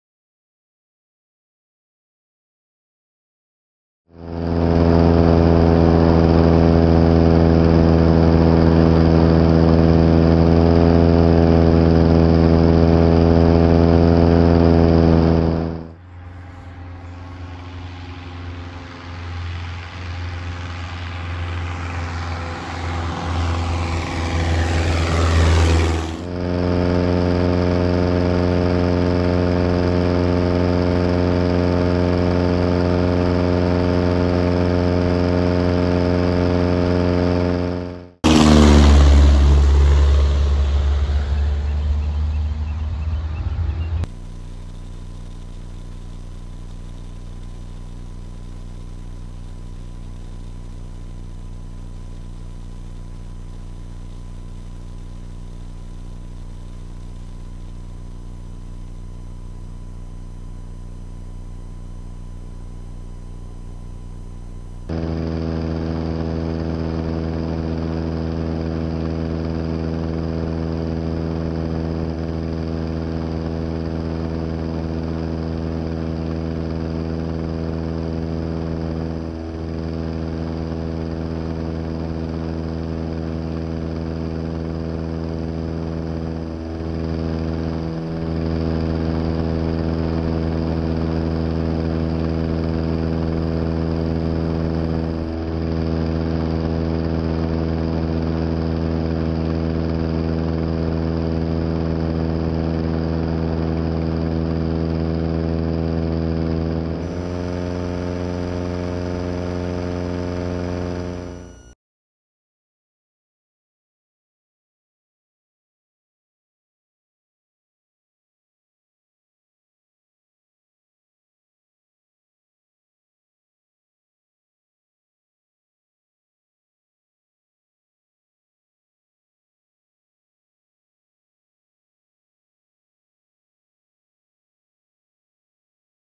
Zenith 801 with Zenair 2500 amphib floats Hartzell C/S prop
This is a video of my first day of flying the 801 with the upgrades (amphibs/CS prop).